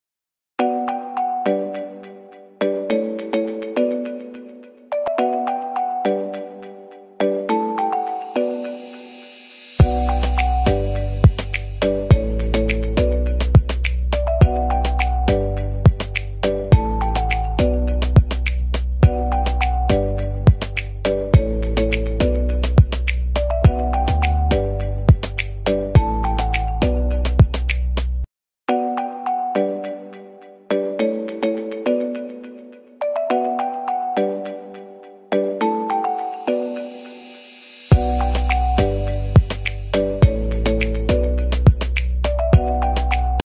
Roaring Tiger Sketch | Pencil Sketch | Charcoal Sketching
The video is about sketching of a roaring tiger and pencils and colour used for the sketching.